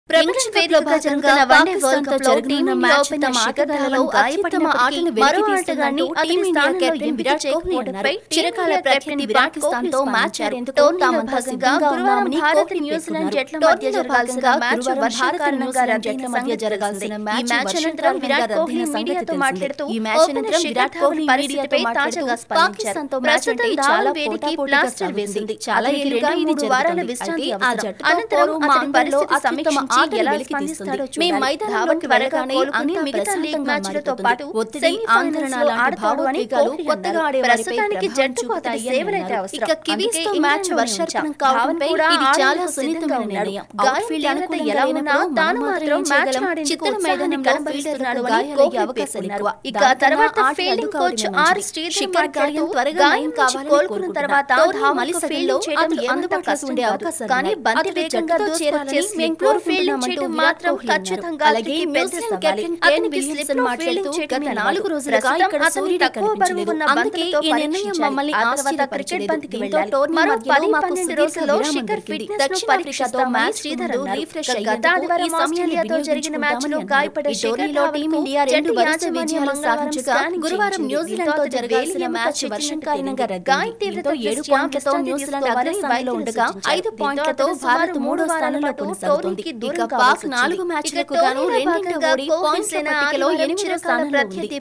ICC Cricket World Cup 2019 : Kohli On Ind V Pak Match After New Zealand Game Abandoned || Oneindia
ICC Cricket World Cup 2019:“Hopefully, the injury heals quickly and he (Dhawan) will be available for the latter half of our league games and semi-finals for sure. From that point of view, we want to hold him back, keep him here,” Kohli said.

ప్రపంచకప్‌లో భాగంగా పాకిస్థాన్‌తో జరగనున్న మ్యాచ్‌ తమ ఆటగాళ్లలో అత్యుత్తమ ఆటను వెలికితీస్తుందని టీమిండియా కెప్టెన్‌ విరాట్‌ కోహ్లీ అన్నాడు. చిరకాల ప్రత్యర్థి పాకిస్థాన్‌తో మ్యాచ్‌ ఆడేందుకు తామంతా సిద్ధంగా ఉన్నామని కోహ్లీ పేర్కొన్నాడు. టోర్నీలో భాగంగా గురువారం భారత్-న్యూజిలాండ్ జట్ల మధ్య జరగాల్సిన మ్యాచ్ వర్షం కారణంగా రద్దైన సంగతి తెలిసిందే.